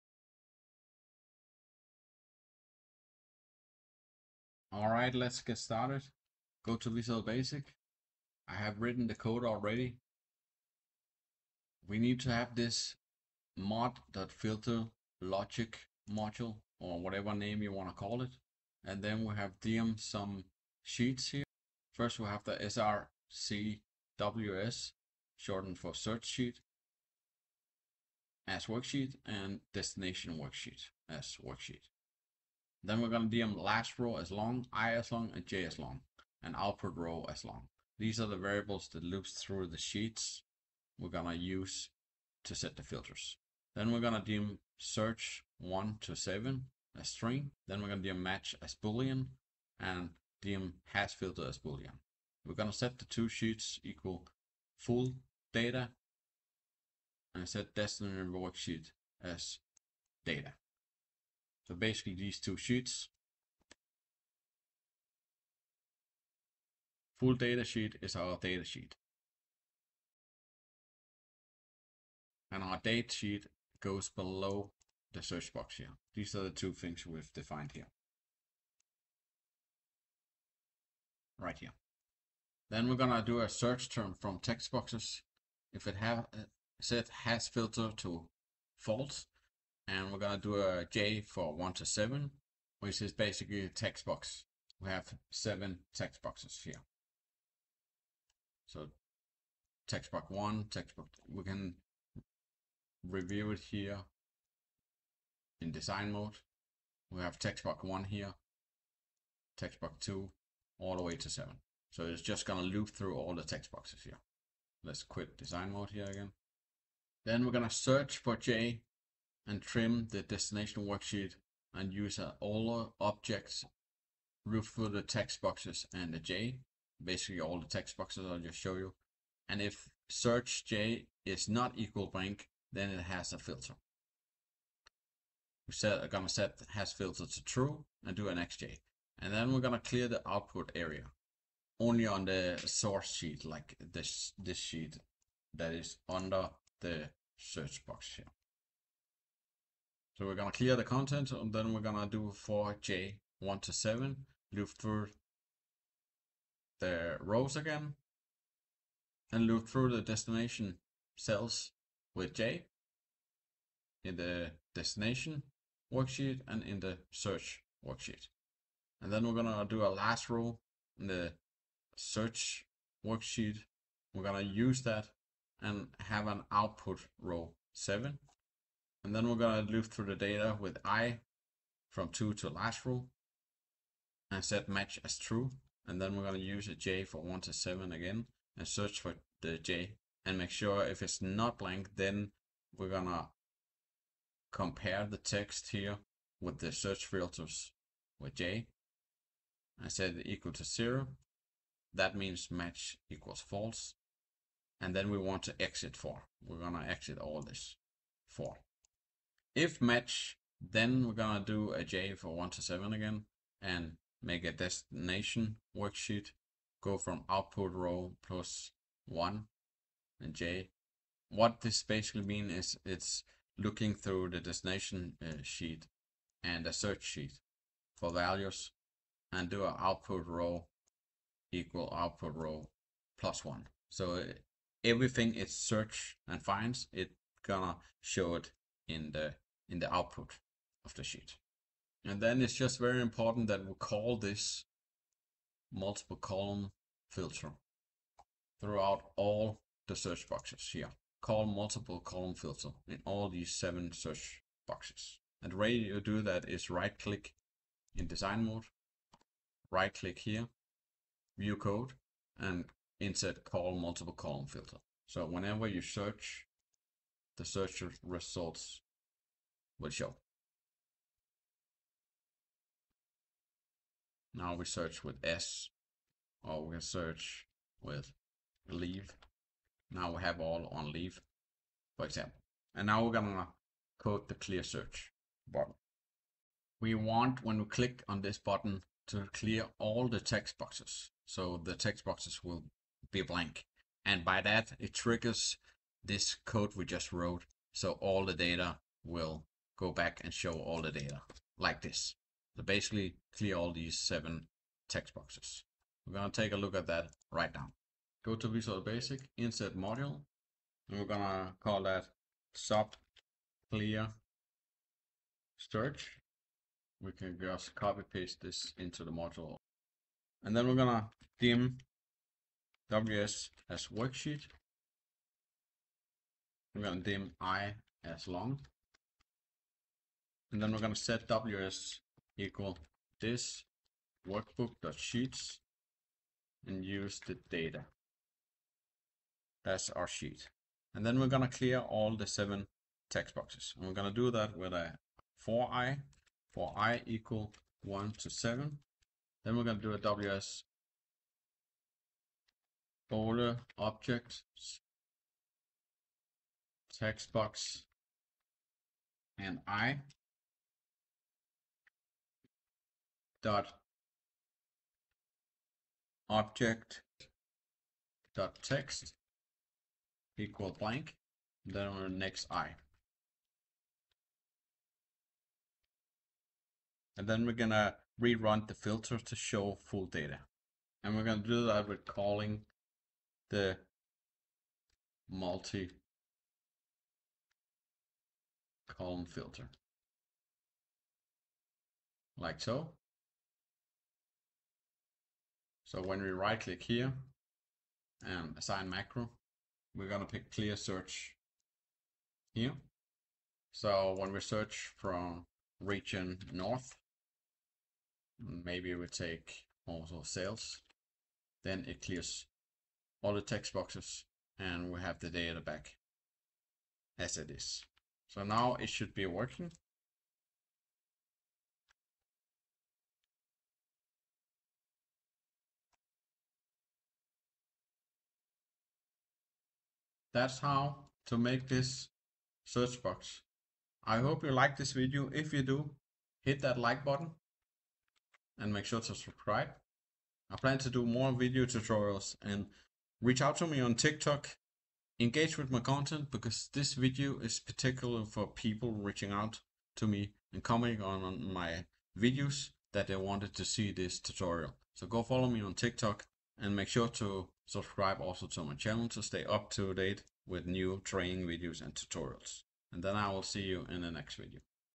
🔍 Create 7-Field Search Filters in Excel with VBA | Step-by-Step Tutorial
In this Excel VBA tutorial, I’ll walk you through how to build a 7-field search box system using TextBoxes and VBA code to dynamically filter data across multiple columns. This is a powerful way to create a searchable interface in Excel without needing any external tools.

You’ll learn:

How to set up a clean user interface with multiple search fields

How to connect TextBoxes to VBA for real-time filtering

How to filter across multiple columns with flexible partial matches

How to separate source data from filtered output for better UX

This method is perfect for anyone who wants to automate data filtering, build lightweight dashboards, or create smart Excel GUIs that save time and reduce manual work.

📲 Follow me for daily content on Excel VBA, SQL, Python, and AI automation: